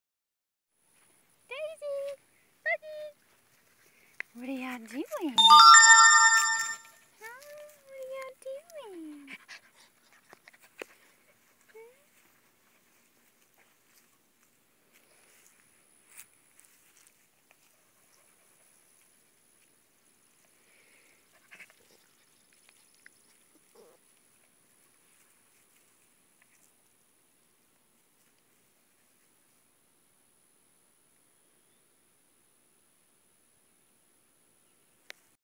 Blind Eskie foster pups from OPAS
Daisy appears to be completely blind and we think Bugsy can see some.  They were surrendered to the Ouachita Parish Animal Shelter in West Monroe, LA by their breeder who works at an area pet store.  This is our Parish's "high kill" public pound and the pups could not be adopted out in their condition.  Although, they have a disability, both are happy pups who get around GREAT!  PAWS of NE LA got them accepted into a rescue in MD and they will be heading North soon.  Until then I get to love on the 2 little cottonballs!!!